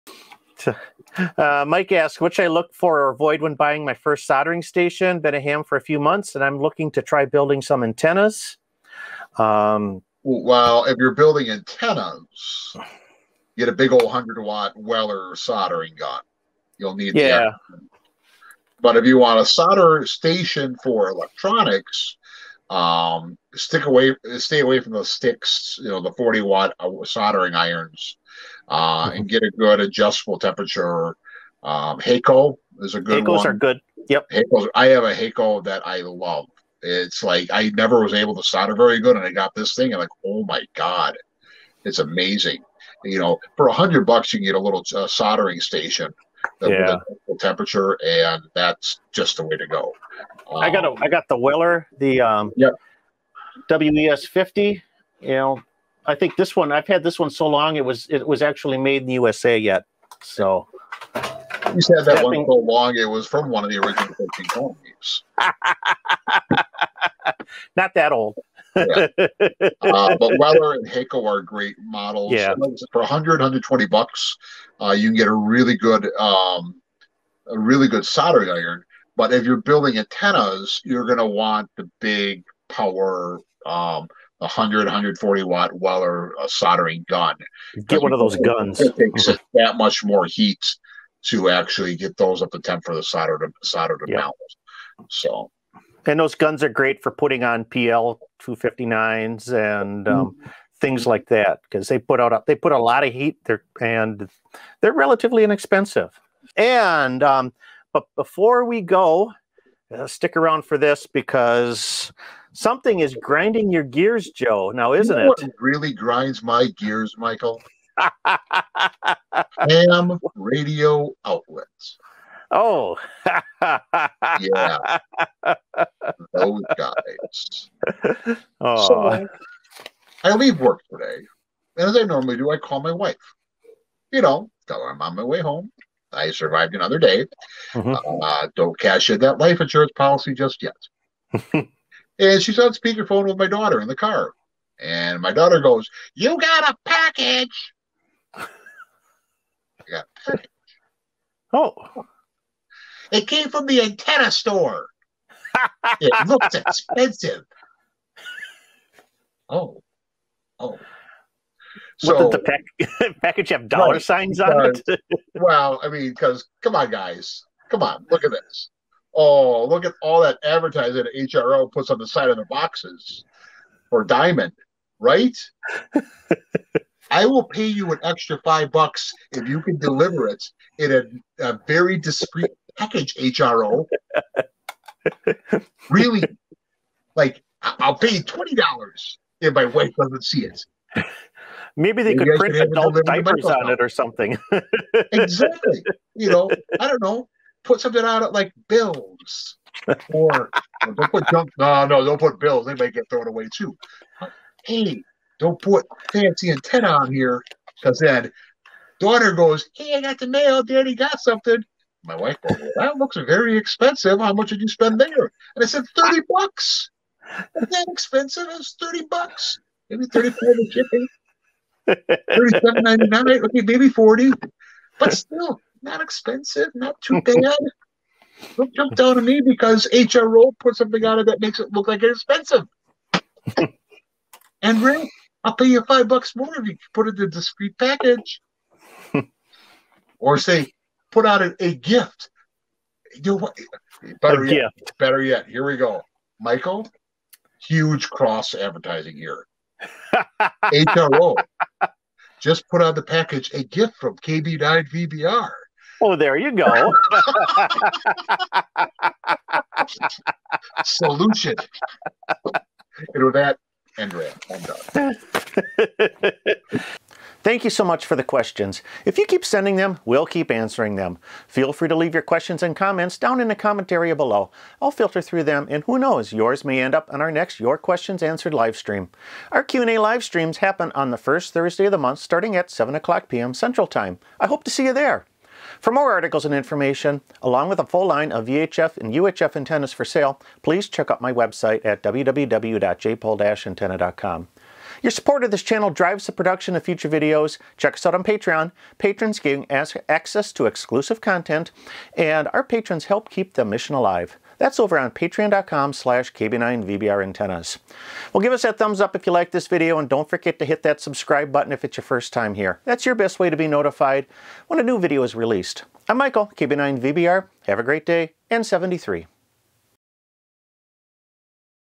Soldering Irons for Antennas (Plus a rant from Joe) July 2021 Livestream Excerpt - Ham Radio Q&A
Our recommendations on soldering irons for antenna work and a rant from Joe about HRO.

Hakko FX888D-23BY Digital Soldering Station
Weller 9400PKS 120V Dual Heat 140/100W Universal Soldering Gun